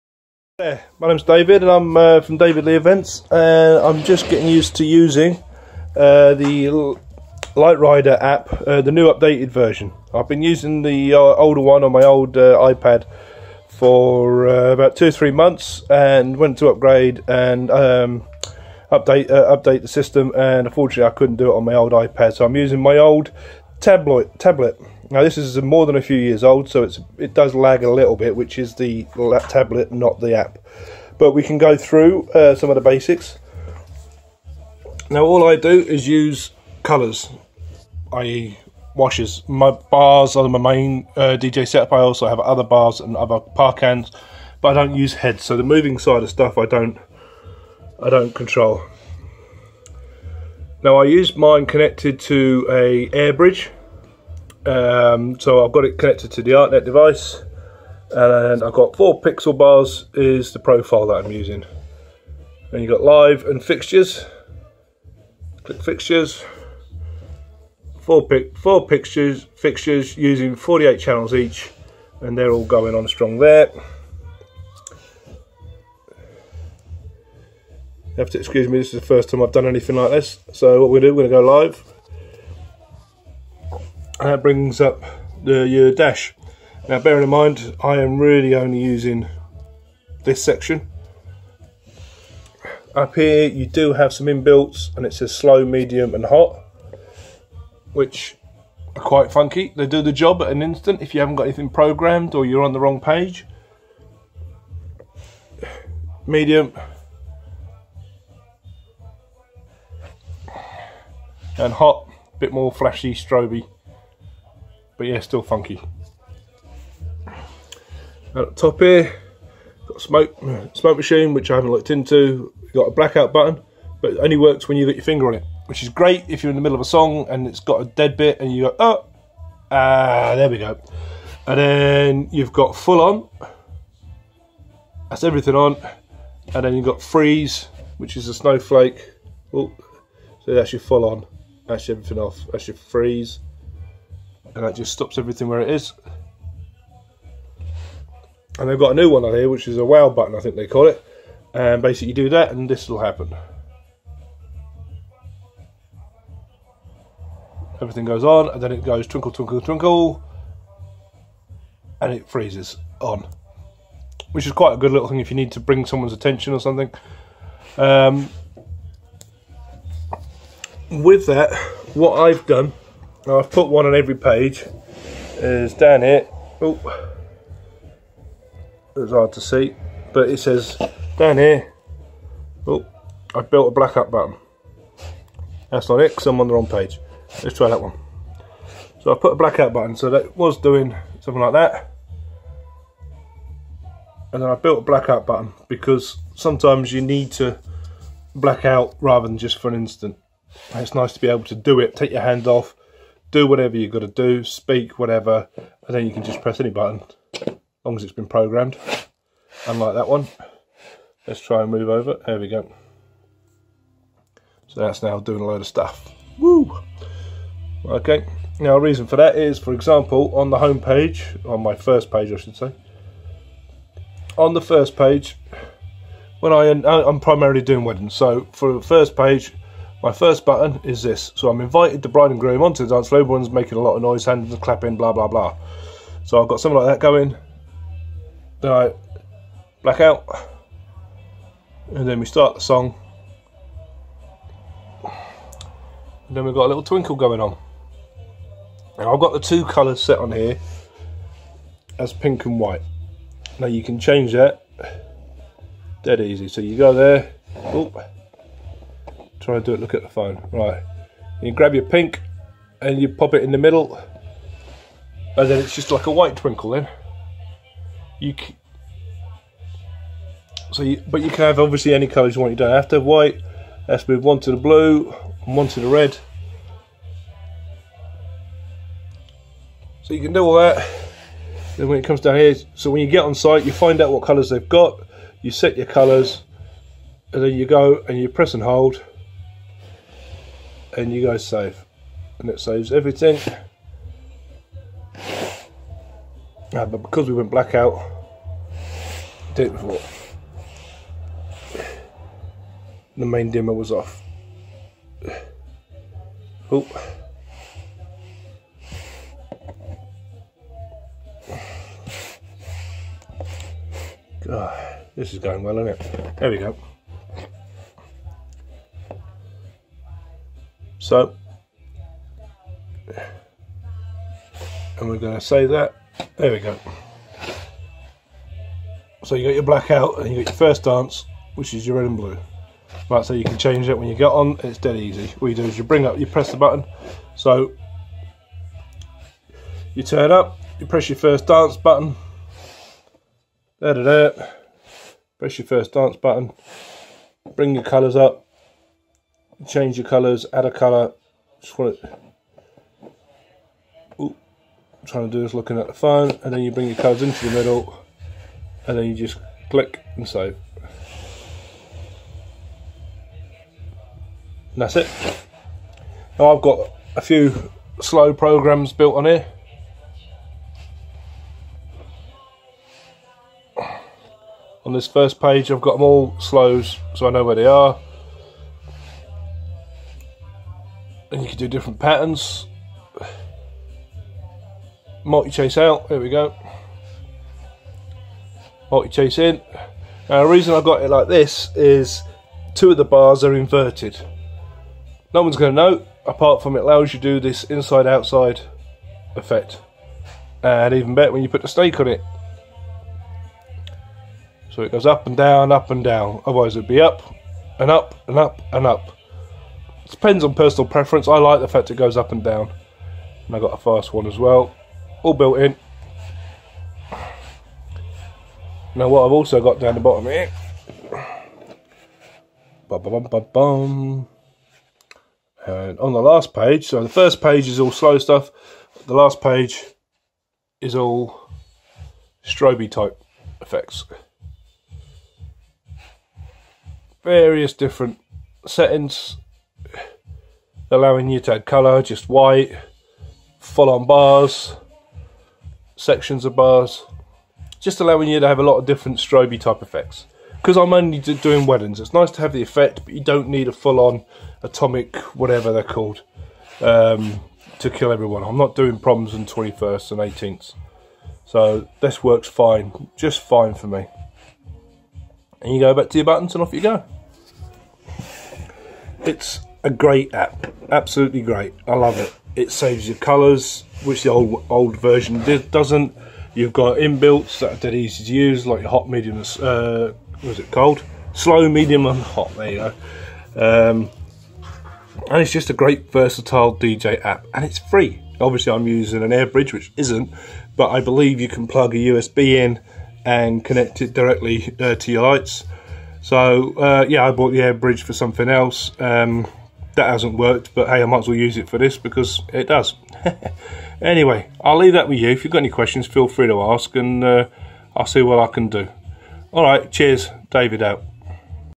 Quick Light Rider app Demo for color effects only.
A quick run through if the Light Rider app on Android Tablet using color bars